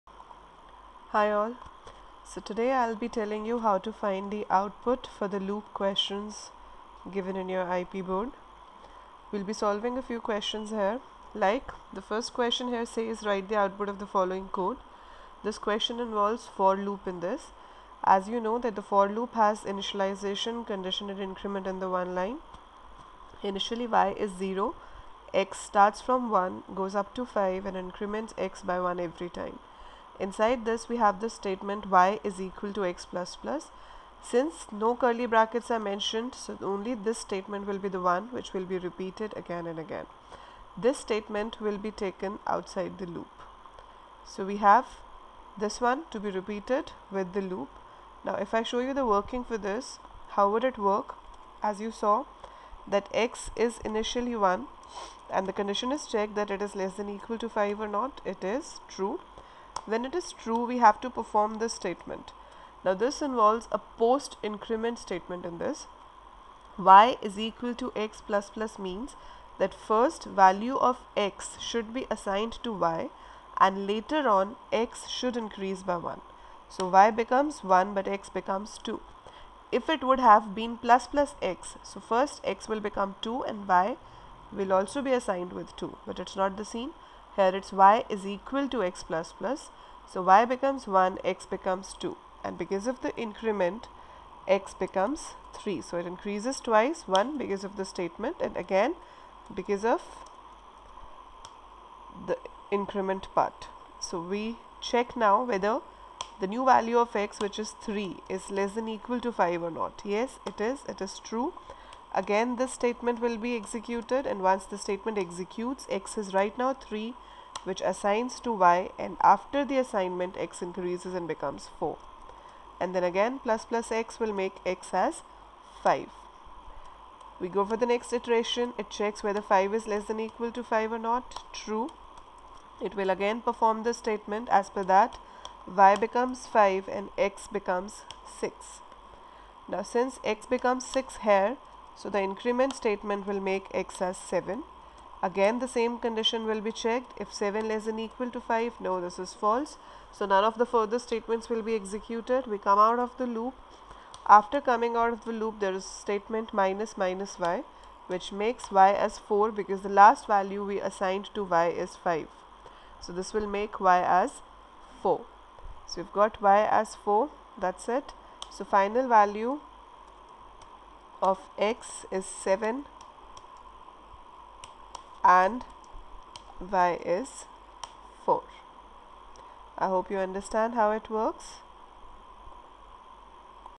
Output Questions XII IP
This is to explain how to find output of the questions of Java class 12th IP Board Exam.